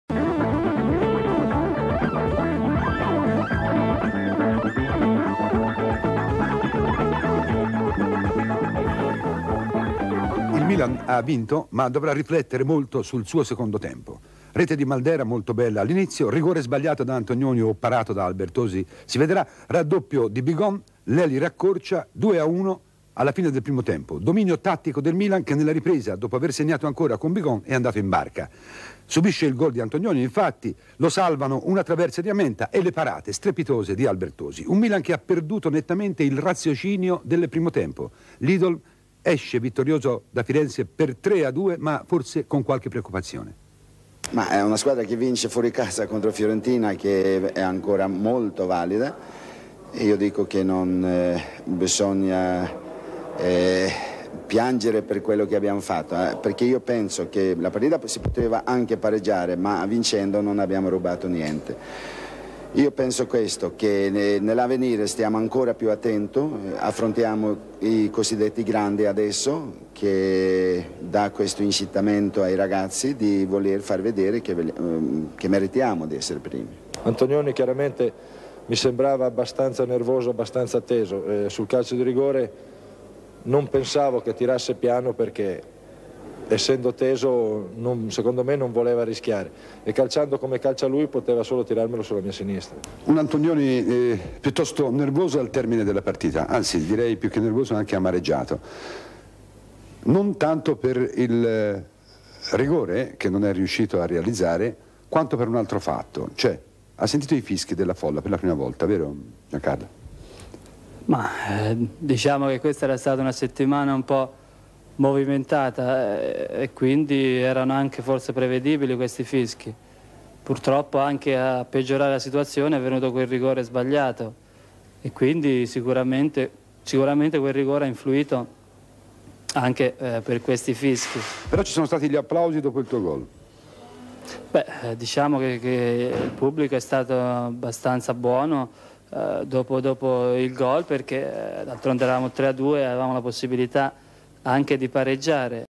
Fiorentina-Milan 2-3 Serie A 78-79 20' Giornata
5'    MALDERA
35'  BIGON
44'  LELY
53'  BIGON
65'  AUT. BARESI